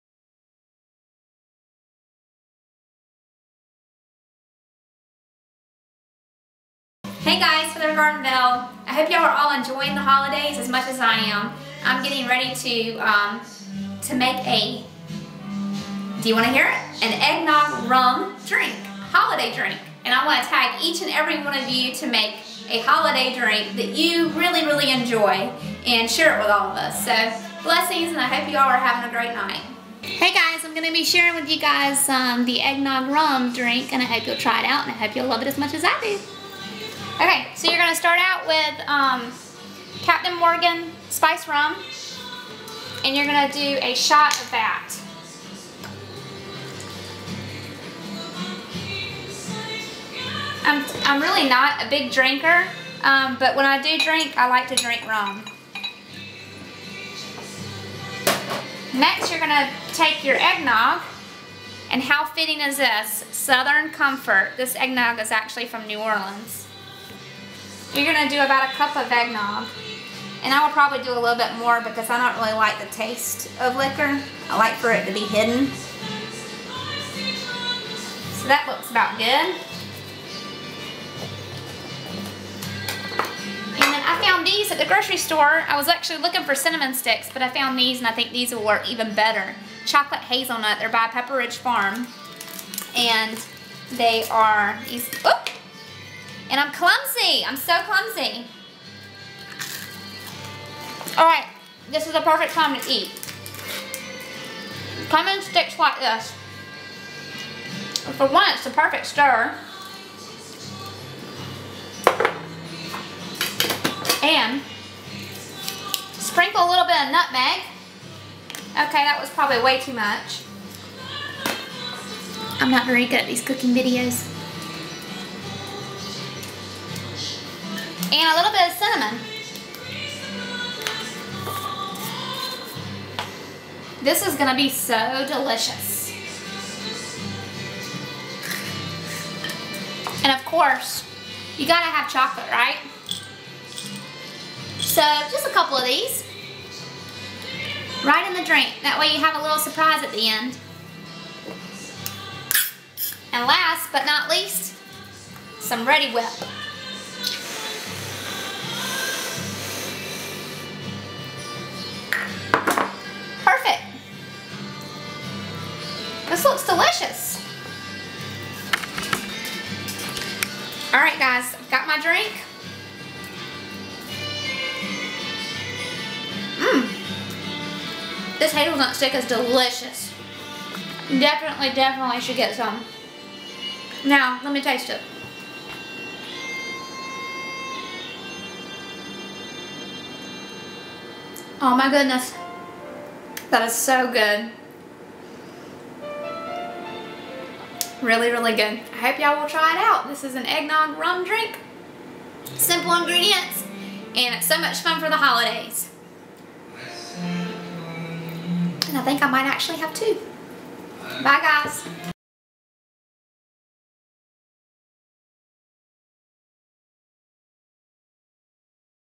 Holiday Drink Tag
What is Your Favorite Holiday Drink?  Tag, You're It!

Eggnog Rum Holiday Drink:
1 Cup of Eggnog
1 Shot of Spicy Rum
Sprinkle of Nutmeg
Sprinkle of Cinnamon
Milk Chocolate Morsels (you decide how many)
1 Chocolate Hazelnut Wafer Stick

Enjoy, FeatherGardenBelle